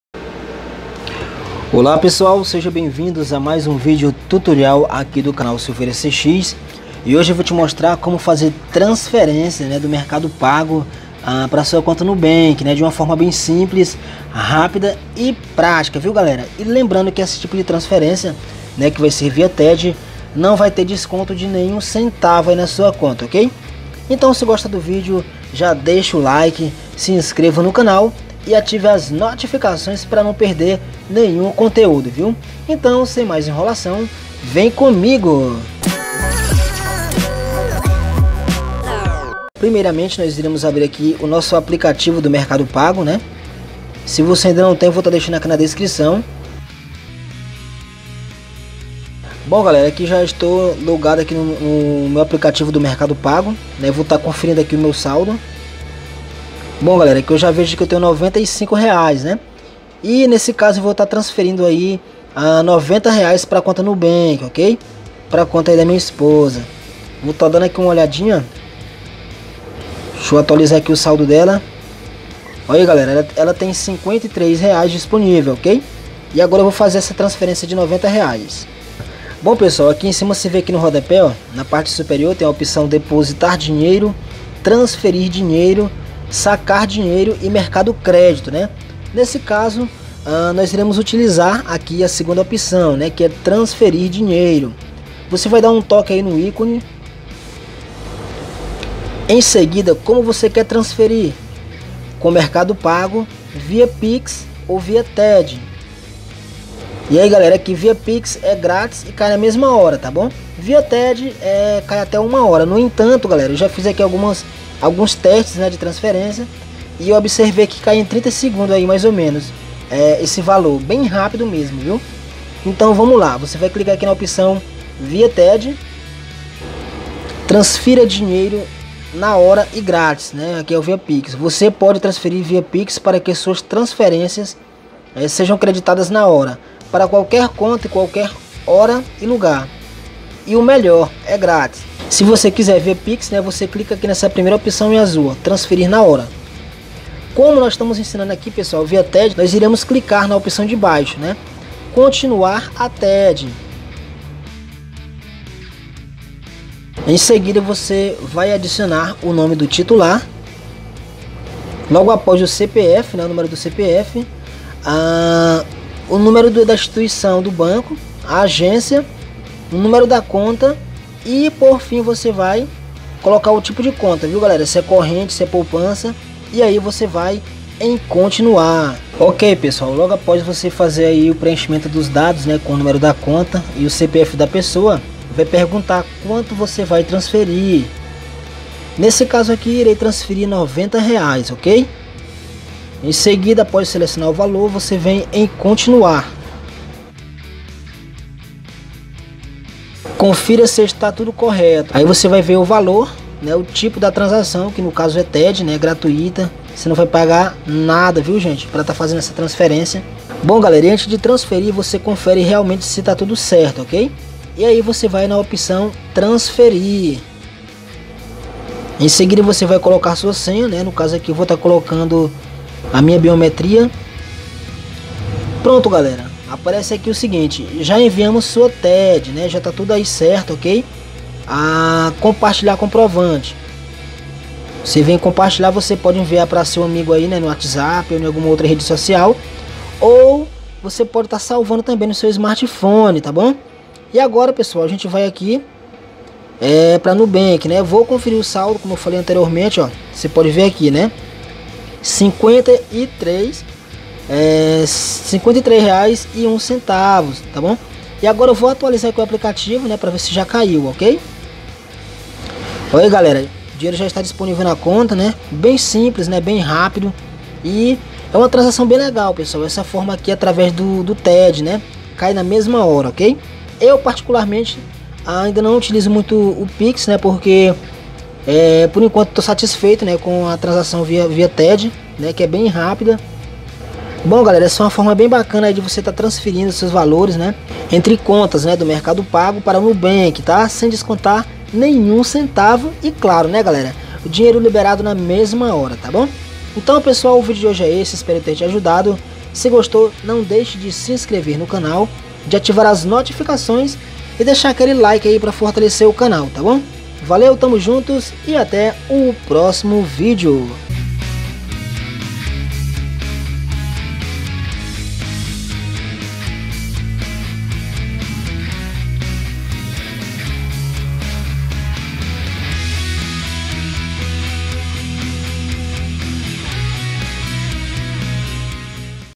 COMO TRANSFERIR DO MERCADO PAGO PARA NUBANK VIA TED CAI NA HORA
MELHOR FORMA DE FAZER TRANSFERÊNCIA ENTRE CONTAS DIGITAIS. EM ESPECIAL DO MERCADO PAGO PARA NUCONTA, VEM PRO PASSO A PASSO...